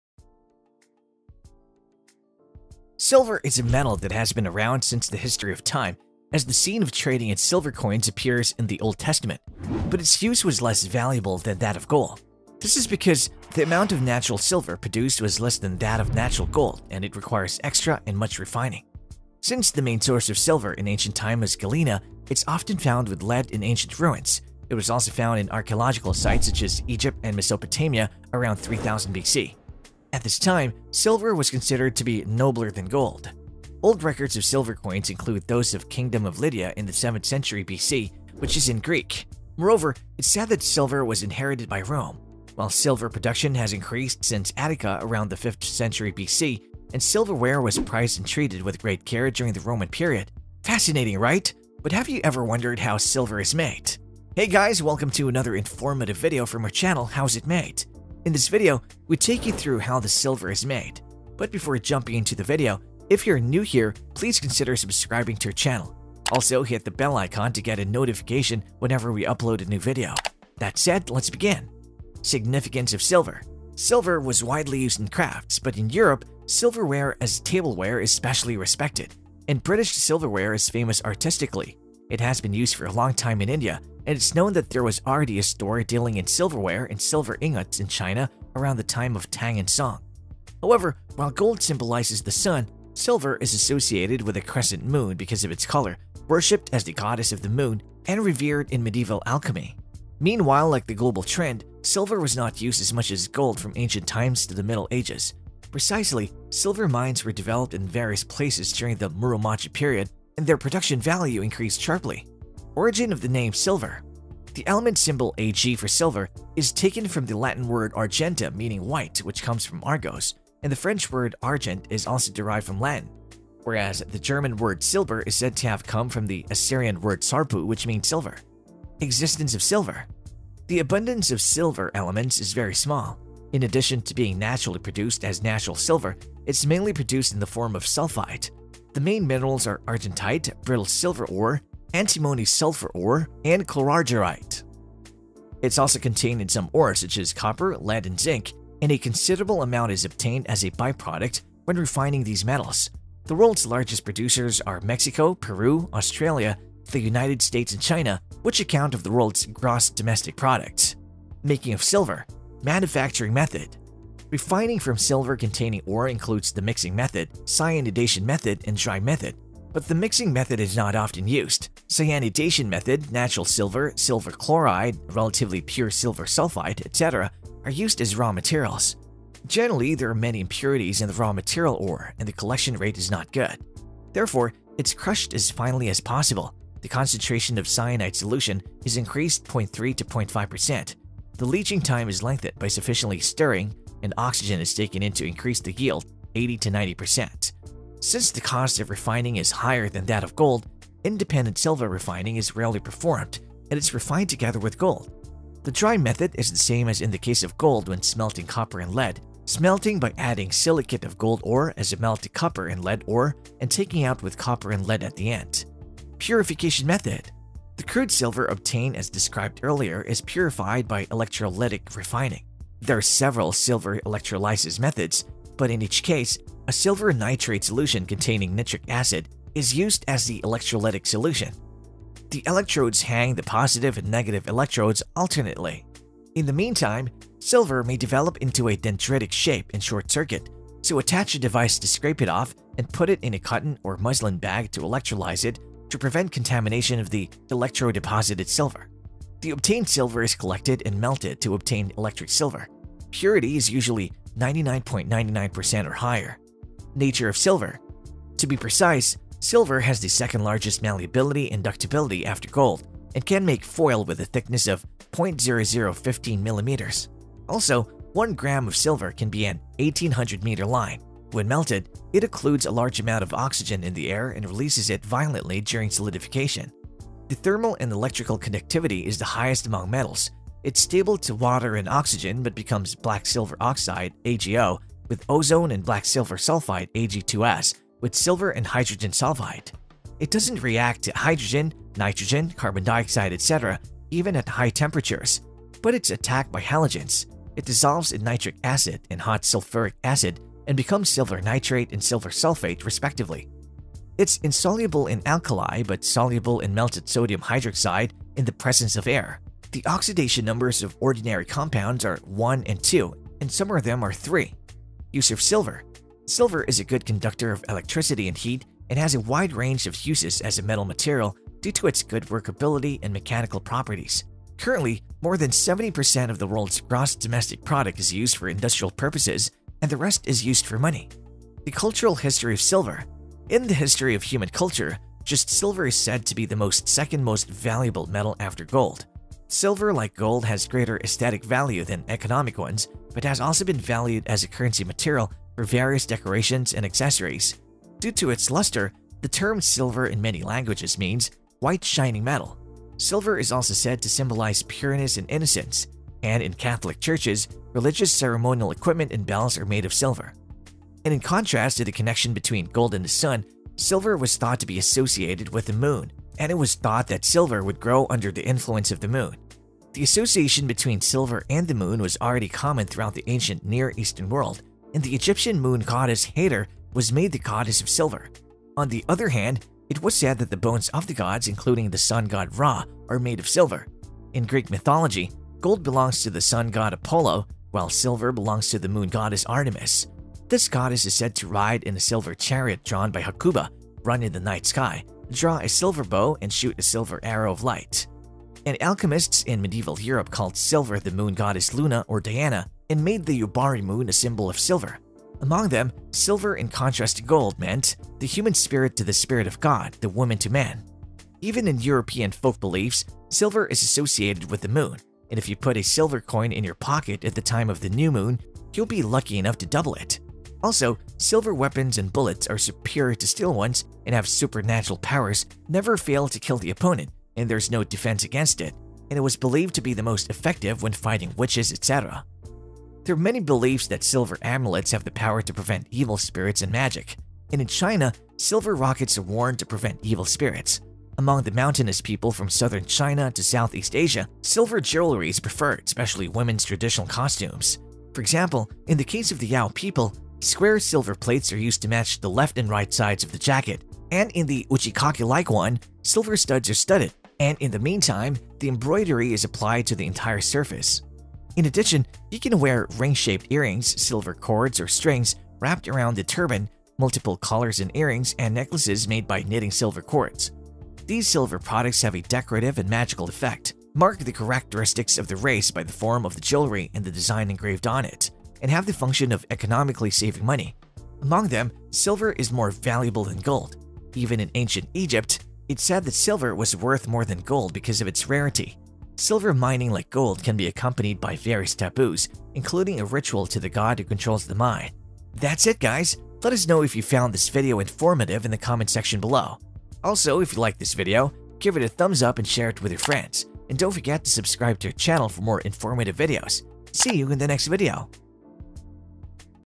Silver in Factories | HOW IT'S MADE
Silver is a metal that has been around since the history of time, as the scene of trading in silver coins appears in the Old Testament, but its use was less valuable than that of gold. This is because the amount of natural silver produced was less than that of natural gold and it requires extra and much refining.

Since the main source of silver in ancient times was galena, it is often found with lead in ancient ruins. It was also found in archaeological sites such as Egypt and Mesopotamia around 3000 BC. At this time, silver was considered to be nobler than gold

Old records of silver coins include those of the Kingdom of Lydia in the 7th century BC, which is in Greek. Moreover, it is said that Silver was inherited by Rome. While, Silver production has increased since Attica around the 5th century BC, and silverware was prized and treated with great care during the Roman period. Fascinating right? But have you ever wondered how Silver is made?

Hey guys! Welcome to another informative video from our channel, How It’s Made. In this video we take you through how the silver is made.

That said, let’s begin!

Significance of Silver
Silver was widely used in crafts, but in Europe, silverware as tableware is especially respected, and British silverware is famous artistically. It has been used for a long time in India, and it is known that there was already a store dealing in silverware and silver ingots in China around the time of Tang and Song.

However, while gold symbolizes the sun, silver is associated with the crescent moon because of its colour, worshipped as the goddess of the moon, and revered in medieval alchemy.

Meanwhile, like the global trend, Silver was not used as much as gold from ancient times to the Middle Ages. Precisely, Silver mines were developed in various places during the Muromachi period, and their production value increased sharply.

Origin of the name: Silver
The element symbol Ag for silver is taken from the Latin word Argentum meaning white, which comes from Argos, and the French word argent is also derived from Latin. Whereas, the German word Silber is said to have come from the Assyrian word sarpu, which means silver.

Existence Of Silver
The abundance of silver elements is very small. In addition to being naturally produced as natural silver, it is mainly produced in the form of sulfide. The main minerals are argentite, brittle silver ore, antimony sulfur ore, and chlorargyrite, and there is also chlorargyrite.

It is also contained in some ores such as copper, lead and zinc, and a considerable amount is obtained as a by-product when refining these metals. The world's largest producers are Mexico, Peru, Australia, the United States and China, which account for most of the world's gross domestic product.

Making of Silver
Manufacturing method
Refining from silver-containing ore includes the mixing method, cyanidation method, and dry method, but the mixing method is not often used. Cyanidation method, Natural silver, silver chloride, relatively pure silver sulfide, etc are used as raw materials. Generally, there are many impurities in the raw material ore, and the collection rate is not so good.

Therefore, it is crushed as finely as possible, the concentration of the cyanide solution is increased (0.3 to 0.5%), the leaching time is lengthened by sufficiently stirring (stirring), and oxygen is taken in to increase the yield (80 to 90%).

Since the cost of refining is higher than that of gold, independent silver refining is rarely performed, and it is refined together with gold. The dry method is the same as in the case of gold when smelting copper and lead, smelting by adding silicate of gold ore as a melt to copper and lead ore and taking out with copper and lead at the end.

Purification method
The crude silver obtained as described earlier is purified by electrolytic refining. There are several silver electrolysis methods, but in each case, a silver nitrate solution containing nitric acid is used as the electrolytic solution. The electrodes hang the positive and negative electrodes alternately.

In the meantime, silver may develop into a dendritic shape and short-circuit, so attach a device to scrape it off and put it in a cotton or muslin bag to electrolyze it to prevent contamination of the electrodeposited silver. The obtained silver is collected and melted to obtain electric silver. Purity is usually 99.99% or higher.